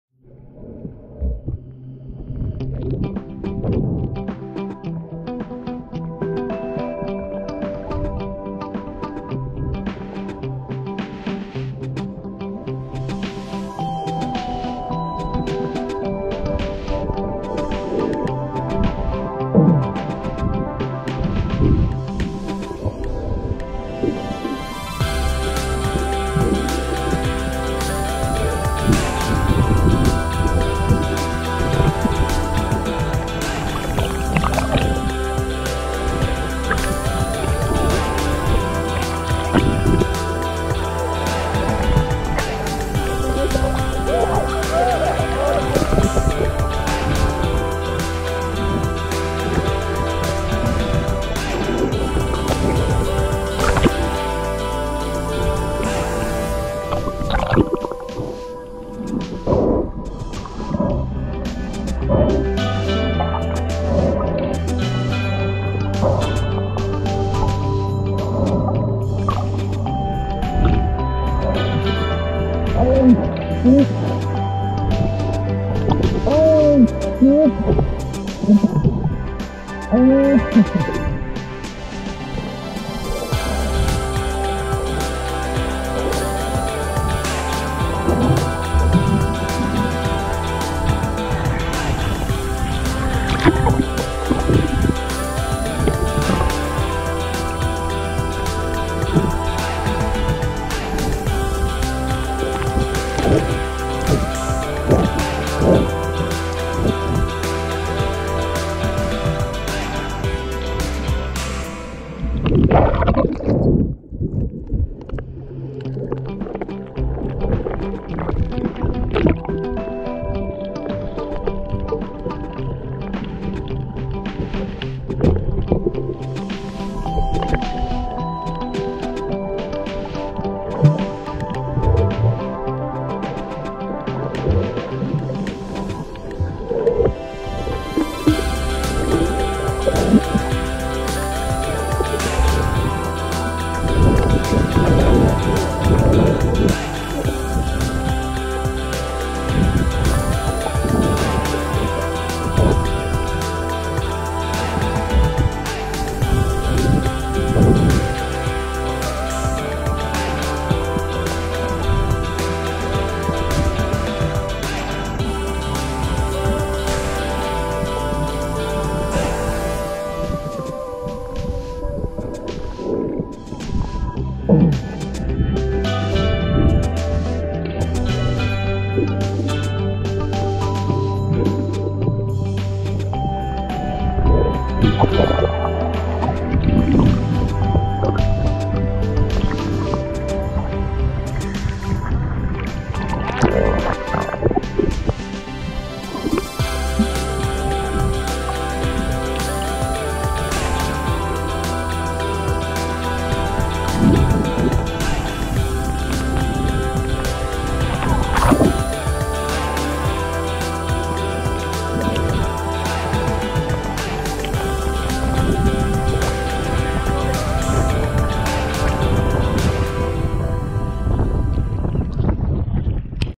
Swimming with sea turtles in Barbados (THE BOAT YARD RESORT BARBADOS 4k)
This is my experience of swimming with sea turtles at the boat yard in Barbados, there were so chilled and allowed us to get up close and personal.